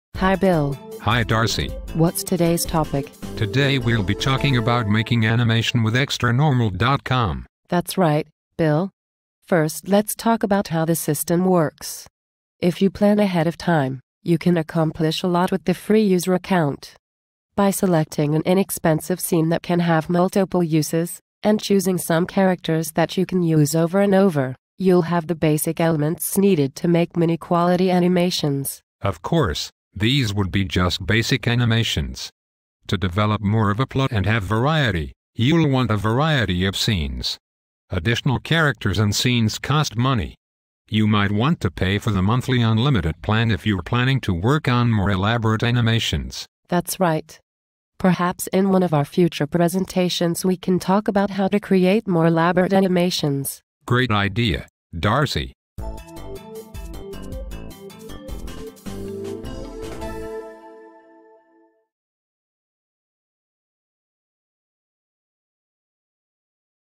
About XtraNormal
A short introduction to XtraNormal animation production.